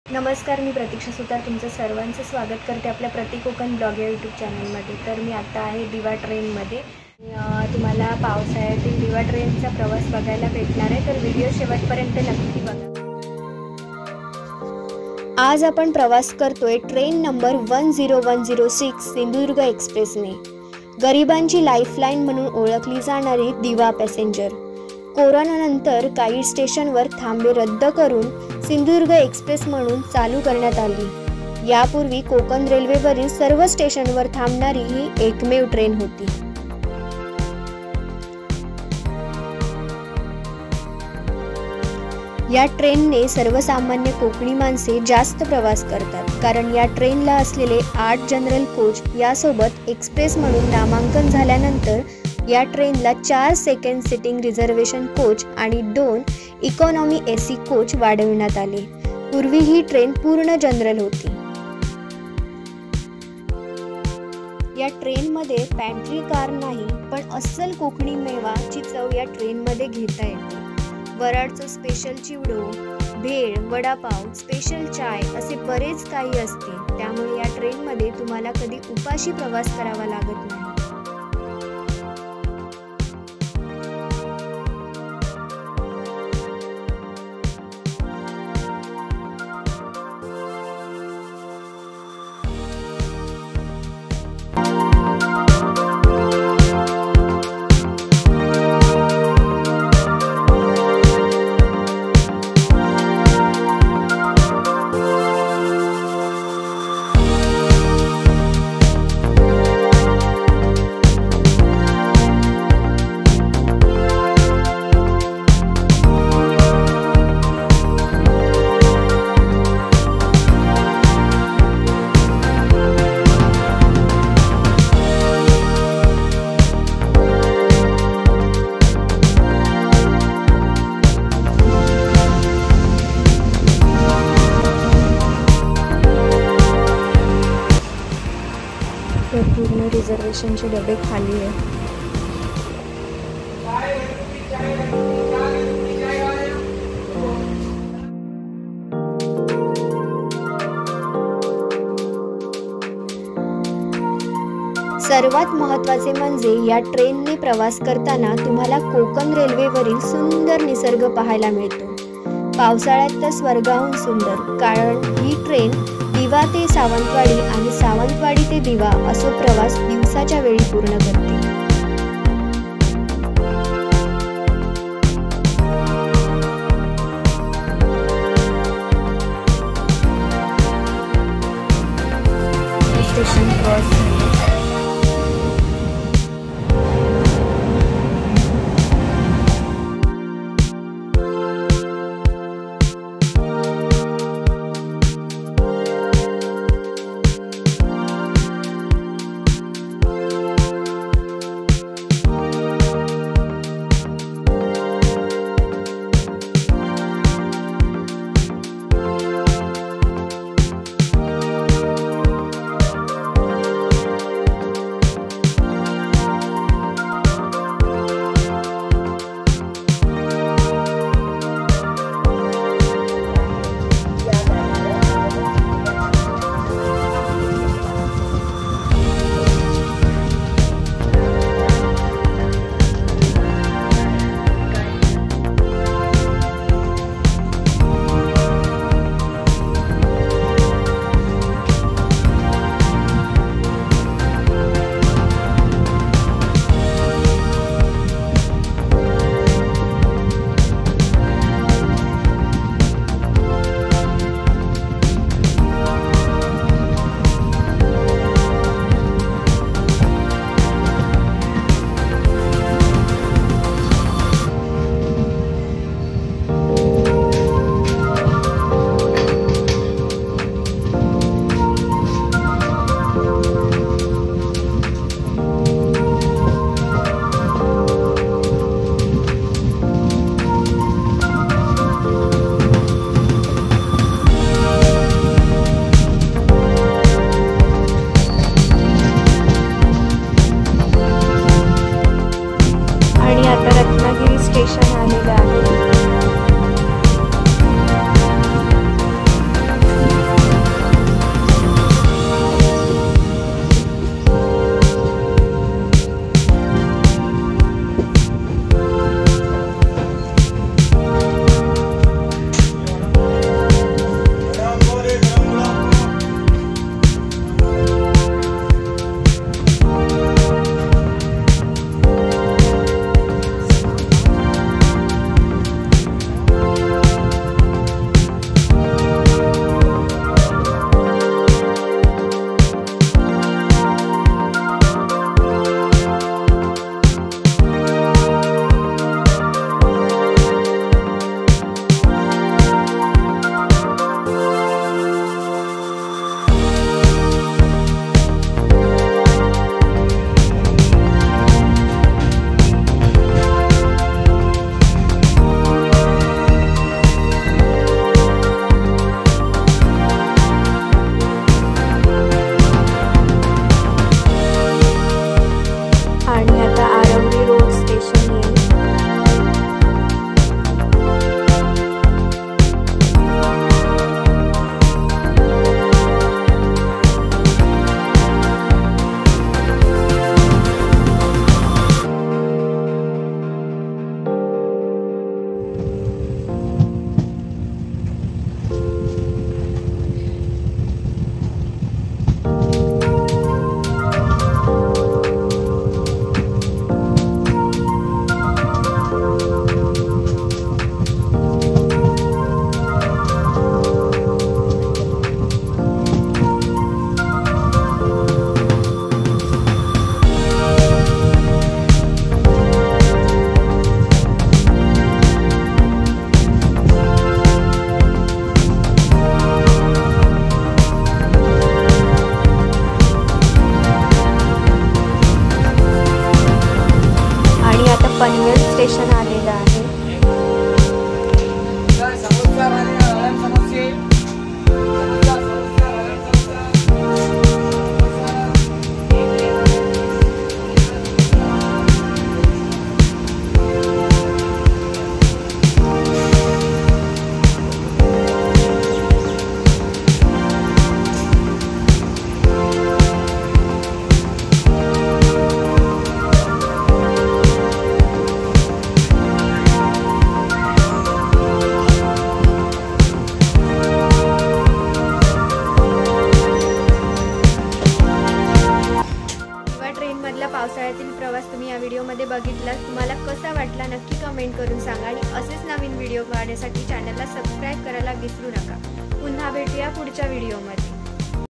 🚂दिवा पॅसेंजर😍पावसाळ्यातील कोकण रेल्वे प्रवास l konkan railway l Travel in monsoon diva passenger
आजच्या व्हिडिओ मधे तुम्हाला दिवा ट्रेन मधील प्रवास बघायला भेटणार आहे. ♥️♥️♥️